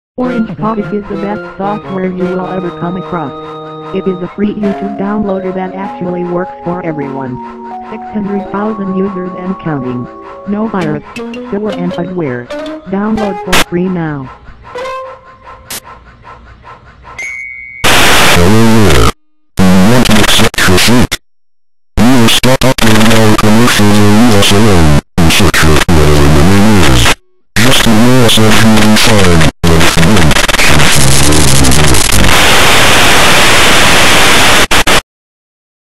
Orange Codec TV Commercial (2010)
HELLOHEREISYOURNICELITTLEGIFT.
THISISOURCOMMERCIALFROM2010.
NO_:MORE,REUPLOADINGOURCOMMERCIALS.
ORSOMETHINGBADWILLHAPPEN.
-10,7cz9dKksl9Oh god, what happened, WHAT HAPPENED??? HE TOOK OVER MY COMPUTER AGAIN! I hate "Orange Codec" so much! You know what? I am gonna ignore him! I am gonna keep uploading the rest of the commercials!"OHYOUHAVECHOSENTHEWRONGOPTION."